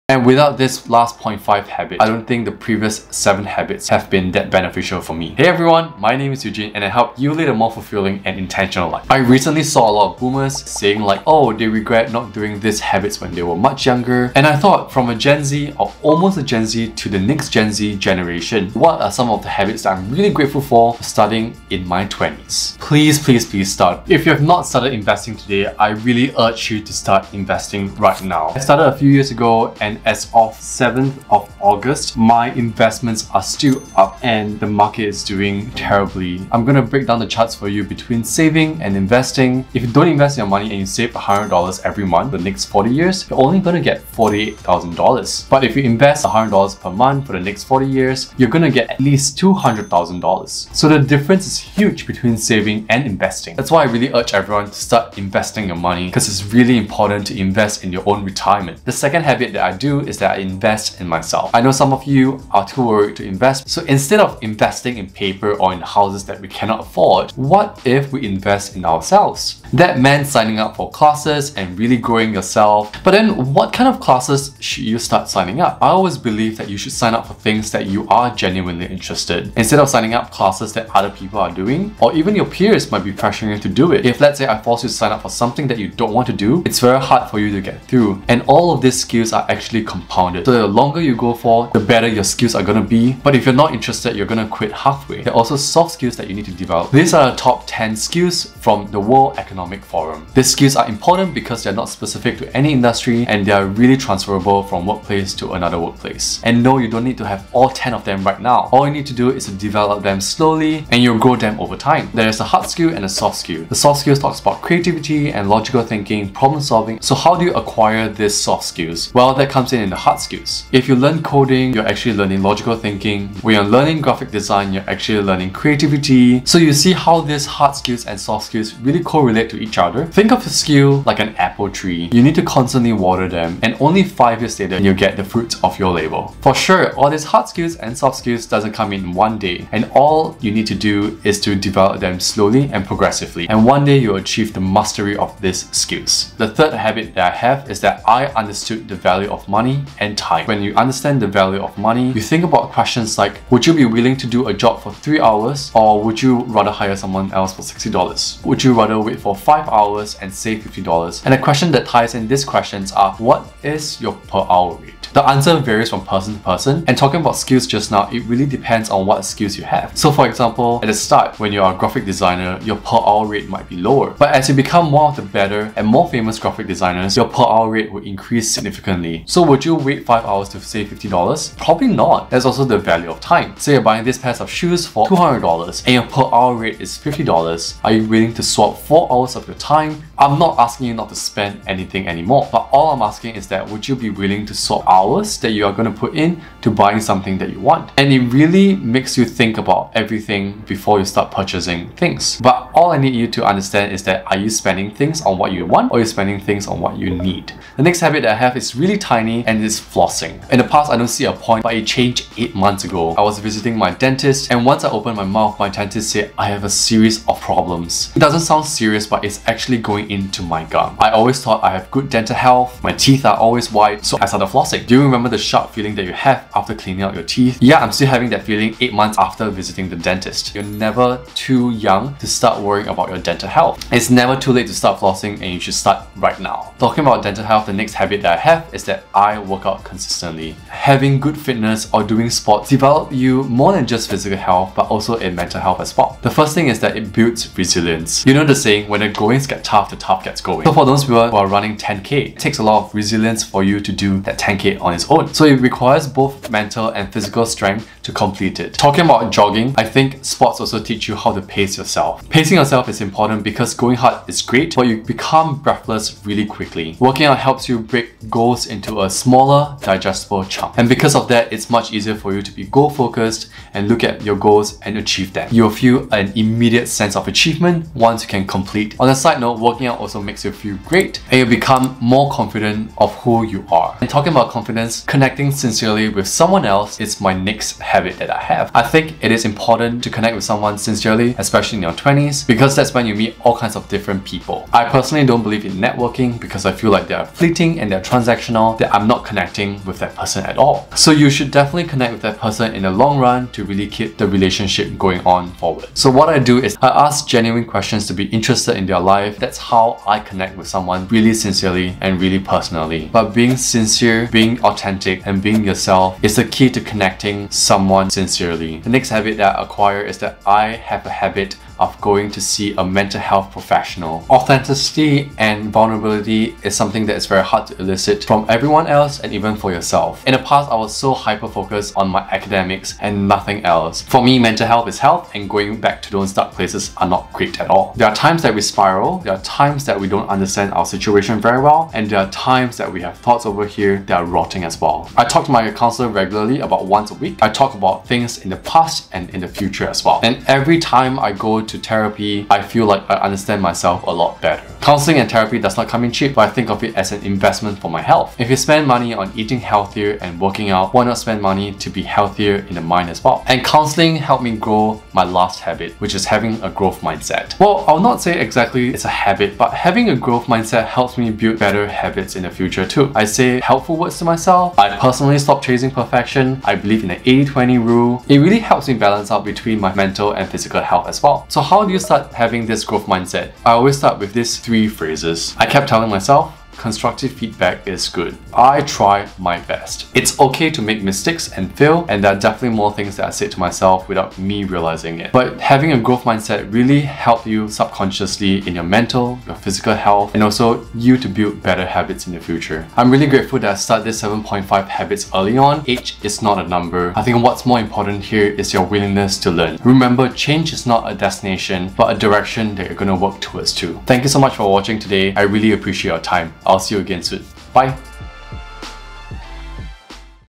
7.5 habits you need to have in your 20s
These are 7.5 habits that I started in my 20s. And ever since I started them, I never looked back. This is beyond just the usual habits, but something to balance my physical and mental health!

Socials:
@tangyble on TikTok/ Twitter/ Instagram!